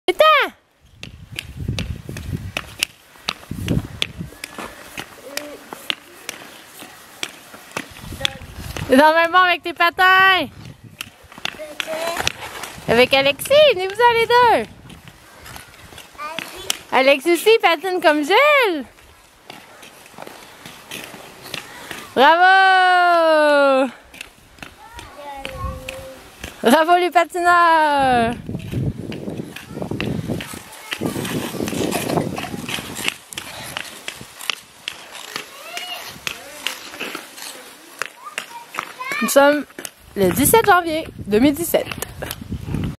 17 janvier 2017 : Ça y est! J'ai réussi à mettre Jules sur ses patins! Et il a eu du plaisir en plus:-)